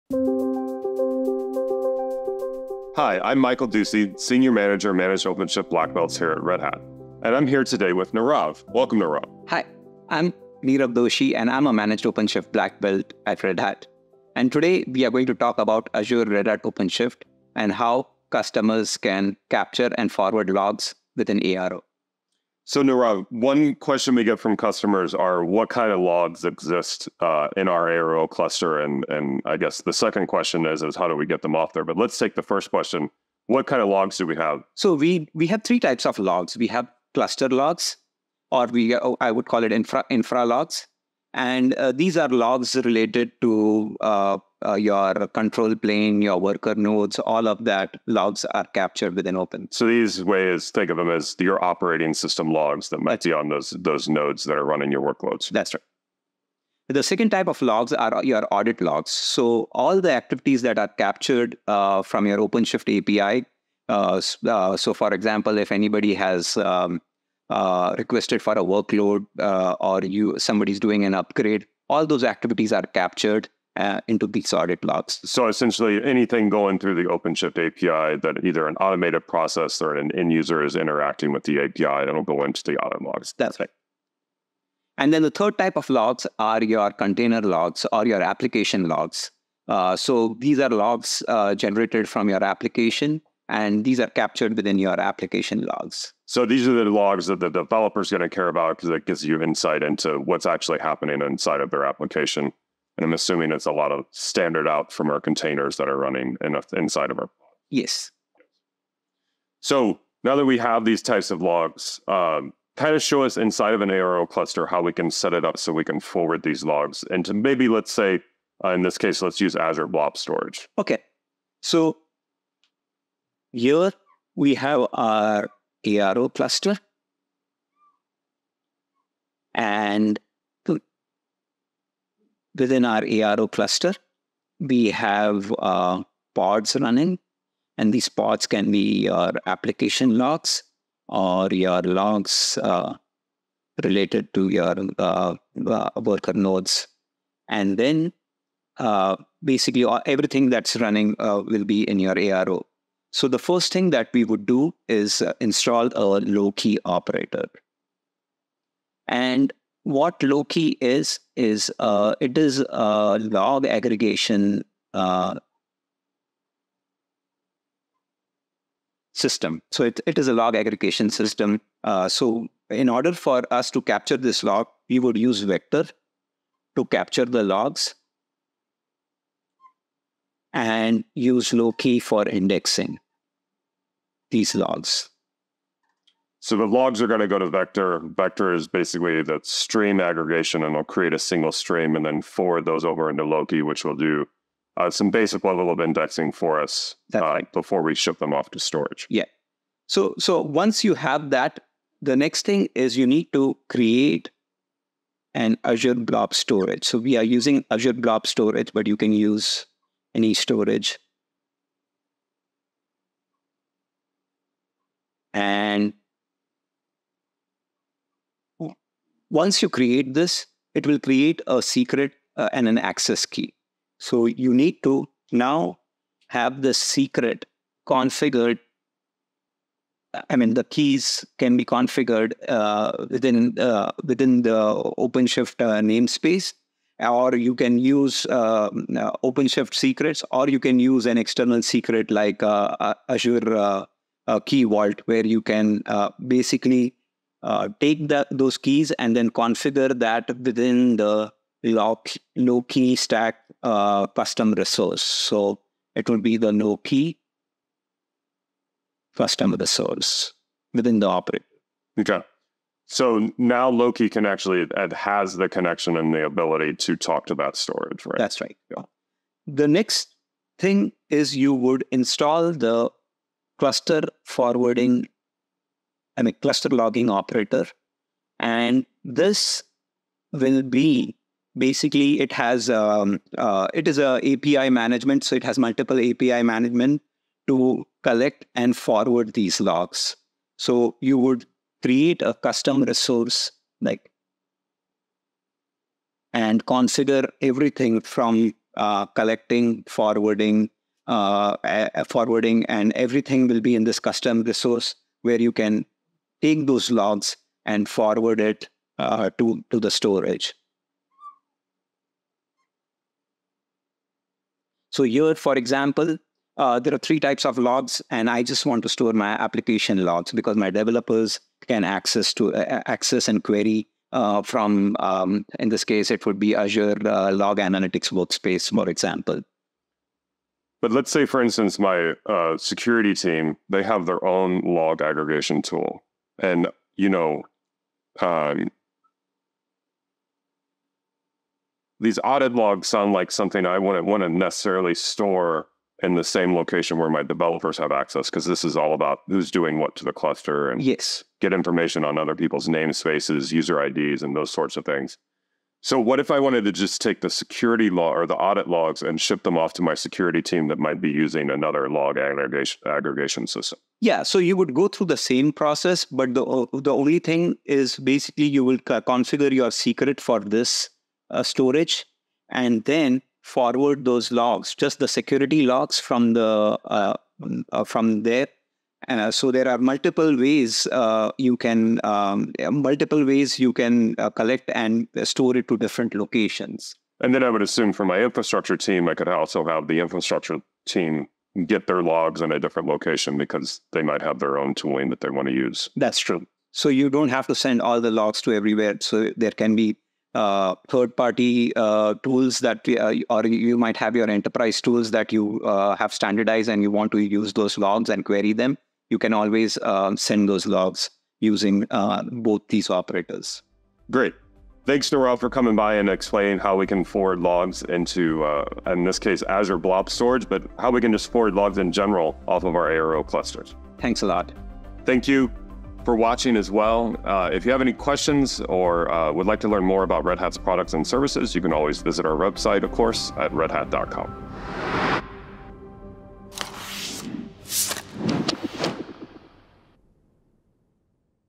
How to forward your logs in Microsoft Azure Red Hat OpenShift to Azure Blob Storage
Michael Ducy, senior manager of the Black Belts, an elite group inside the Red Hat OpenShift team, and Nerav Doshi, senior Black Belt, walk us through the steps and procedures needed to setup blob storage on Microsoft Azure Cloud to receive log files from Microsoft Azure Red Hat OpenShift.